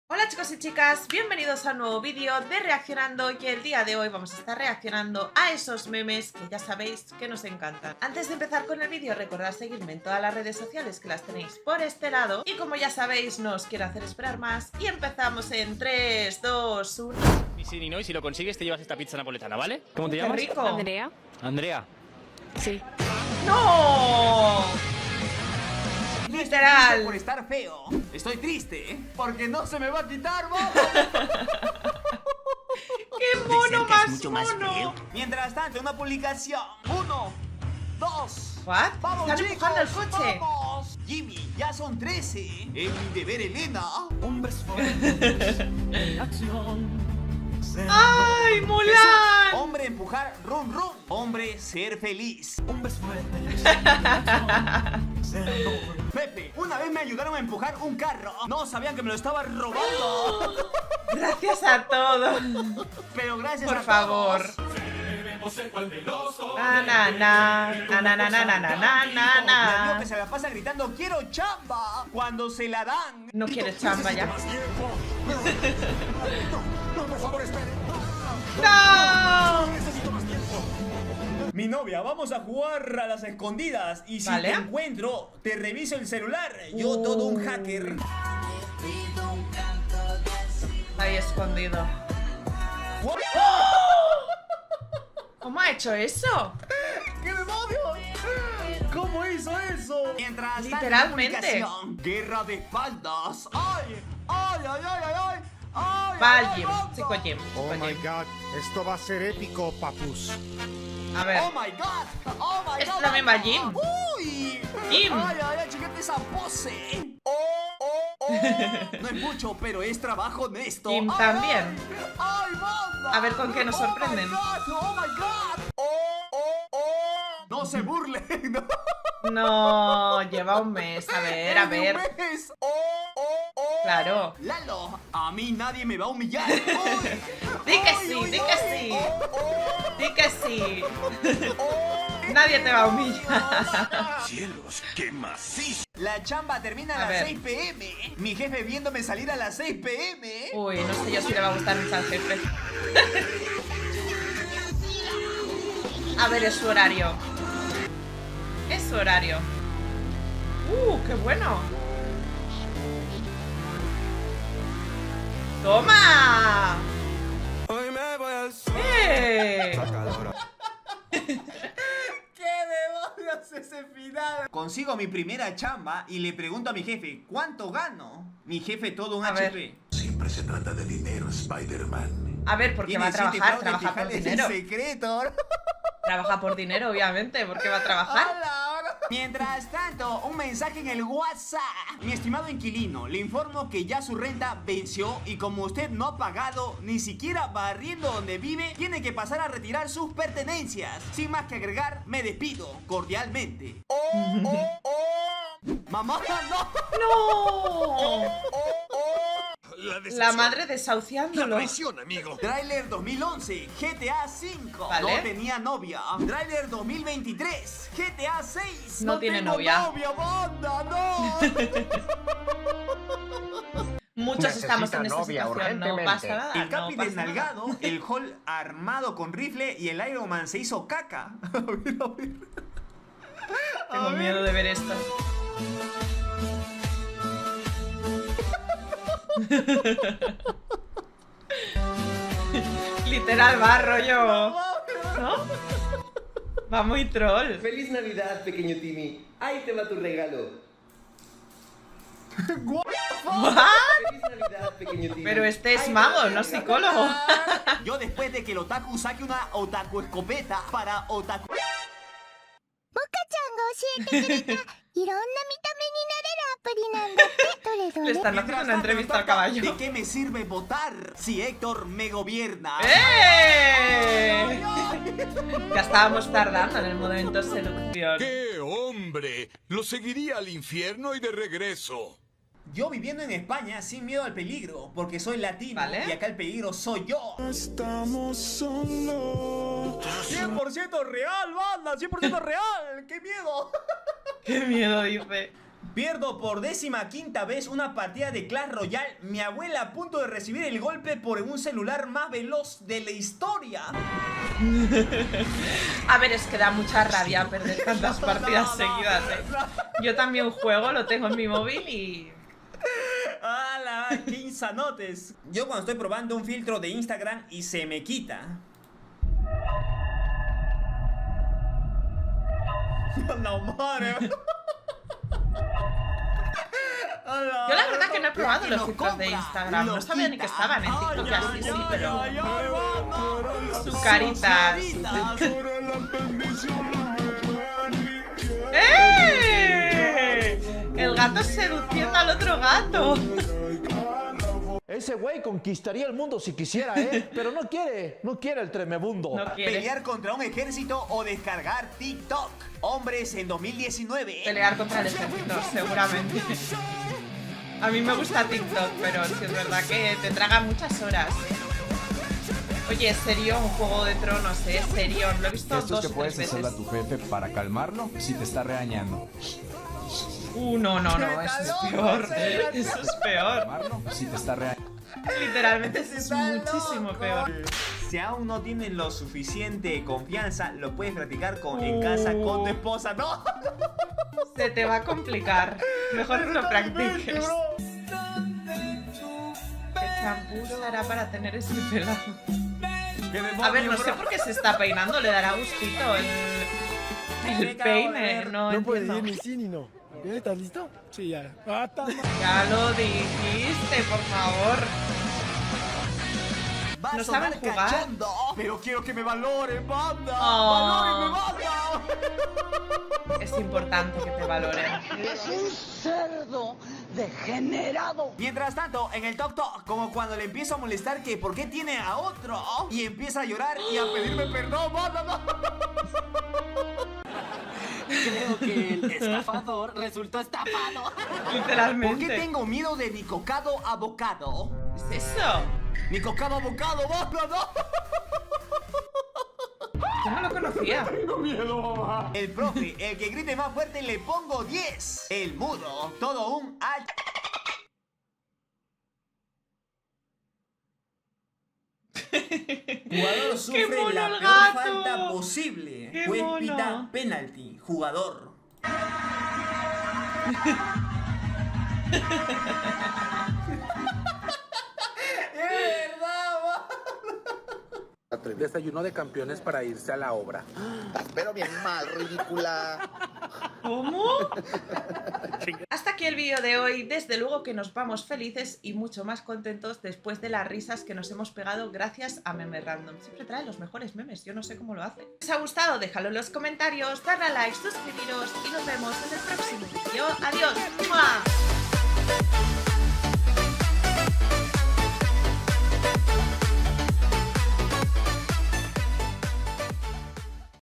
Reaccionando a los mejores MEMES RANDOM | NO PODRAS PARAR DE REIR 😂
Reaccionando a los memes random más graciosos en español.